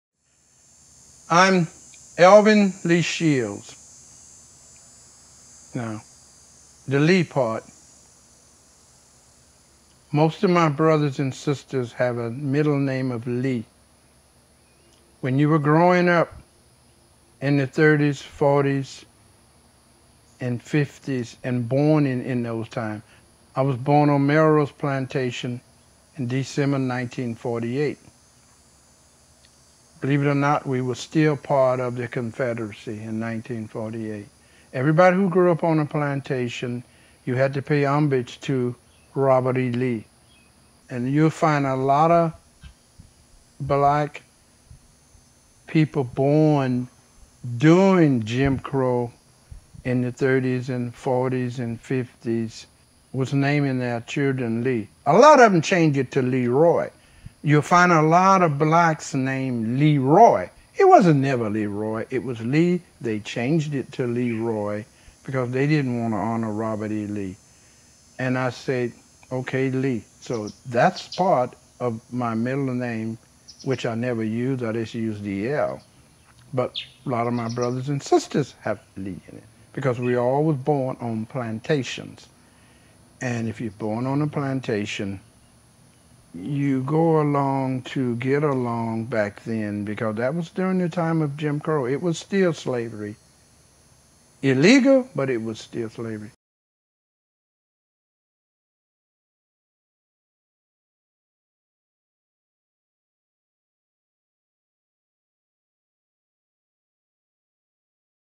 A Preview of Personal Recollections of the Plantation with Elvin Shields
This is a preview to our Personal Recollection of the Plantation with Elvin Shields video. Here is the story of Mr. Shields' name and how many others born of the same time share this same name.
The full version can be found under our Cane River Tenant Cabins playlist along with other related videos!